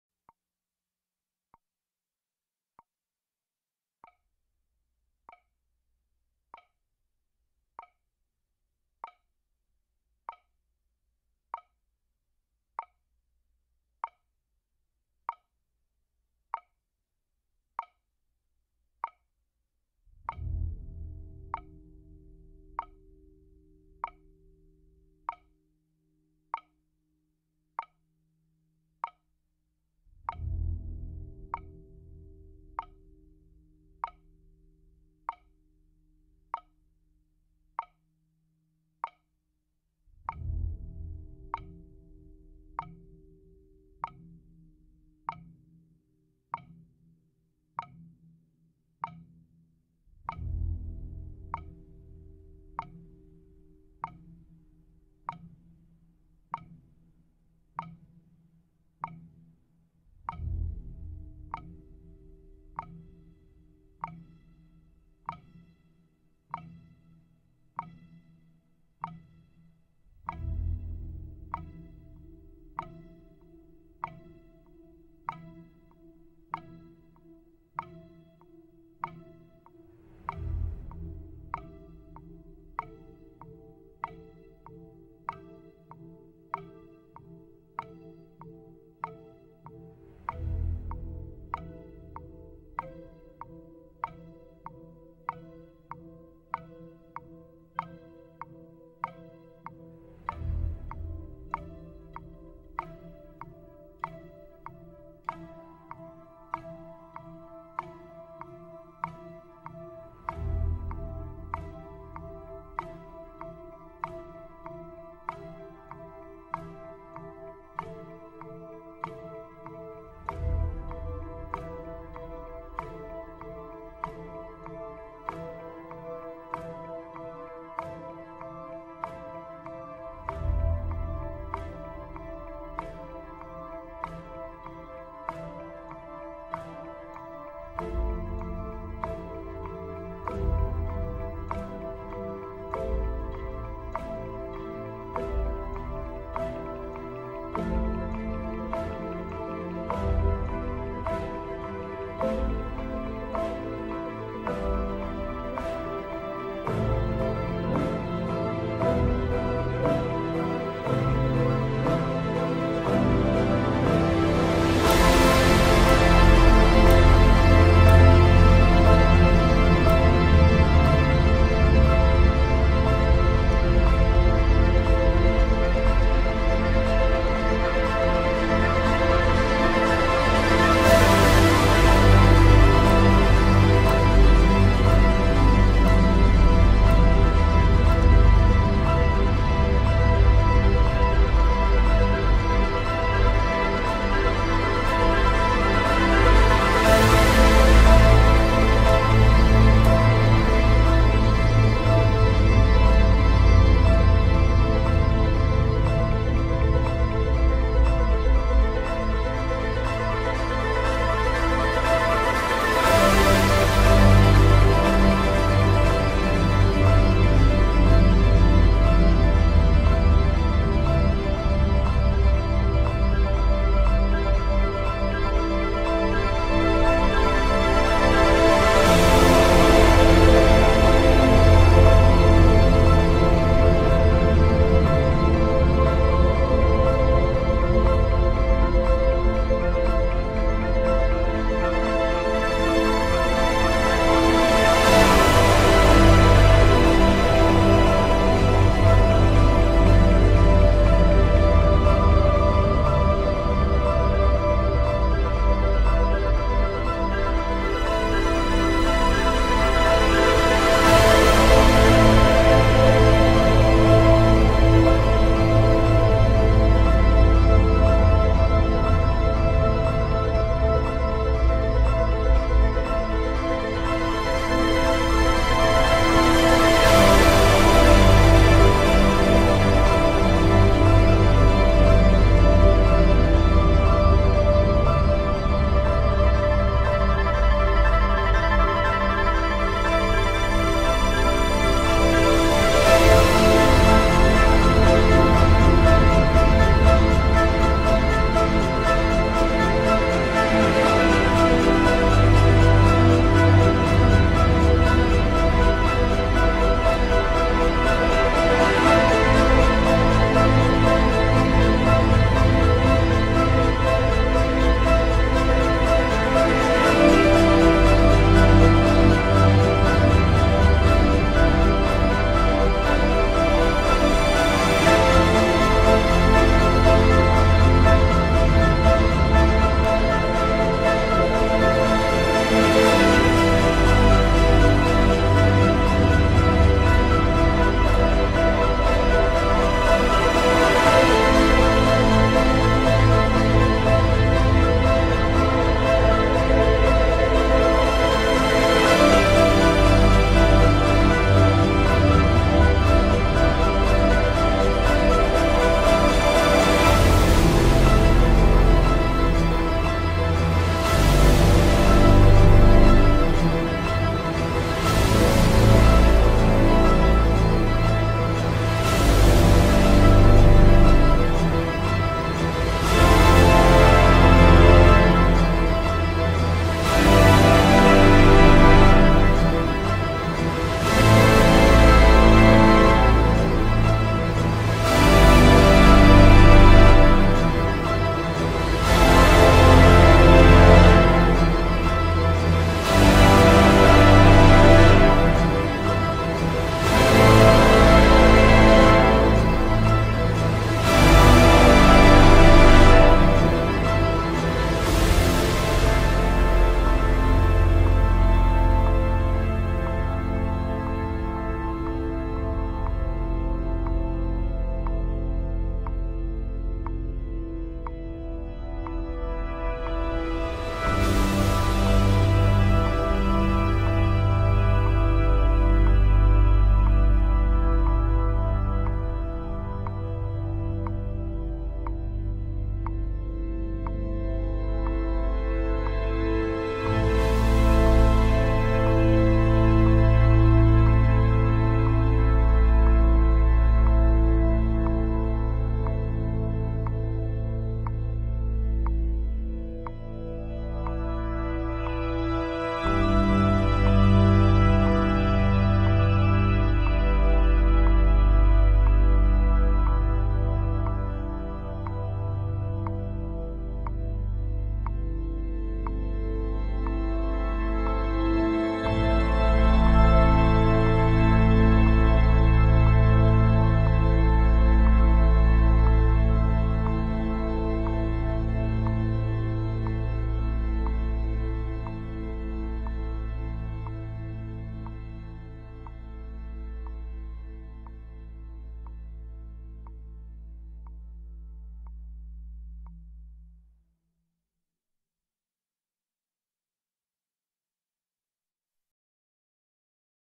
2023MaladMusic
The Malad Gorge from Put in to just above the snake. Music by Han Zimmer